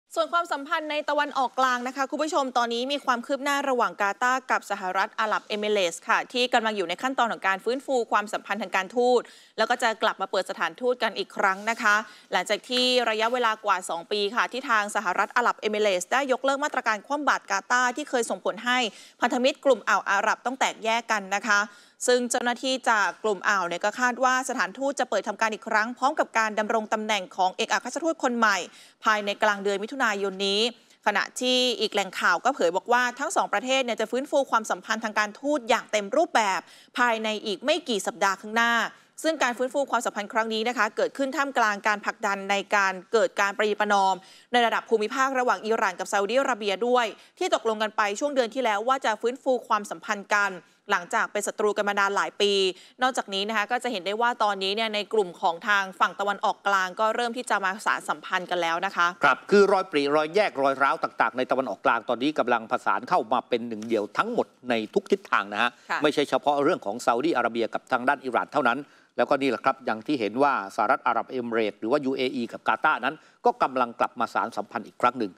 โลกเปลี่ยนแล้ว! UAE-กาตาร์ จับมือสงบศึก หลังบาดหมางนานหลายสิบปี I ผ่าประเด็นโลก
ตะวันออกกลางขยับ โลกเปลี่ยนแล้ว! UAE-กาตาร์ เตรียมจับมือสงบศึก หลังบาดหมางมานานหลายสิบปี